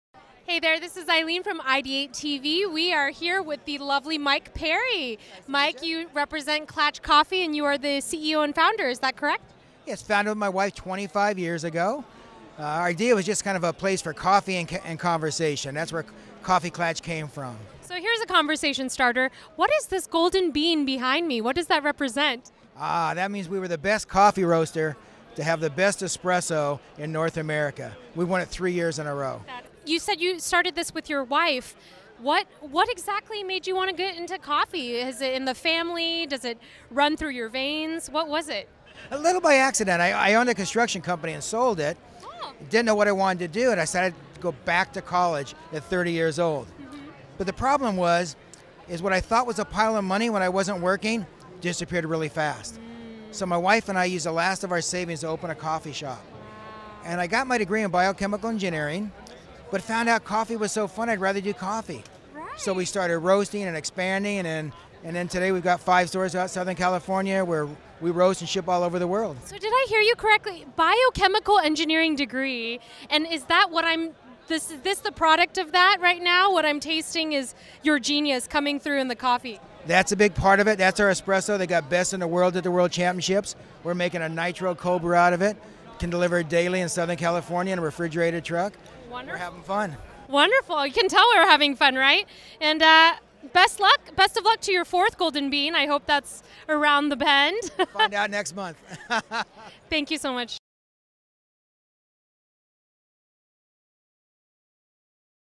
Klatch Coffee - Coffee Fest 2018
Klatch Coffee was interviewed at the Coffee Fest in 2018. We had such a wonderful time at this event.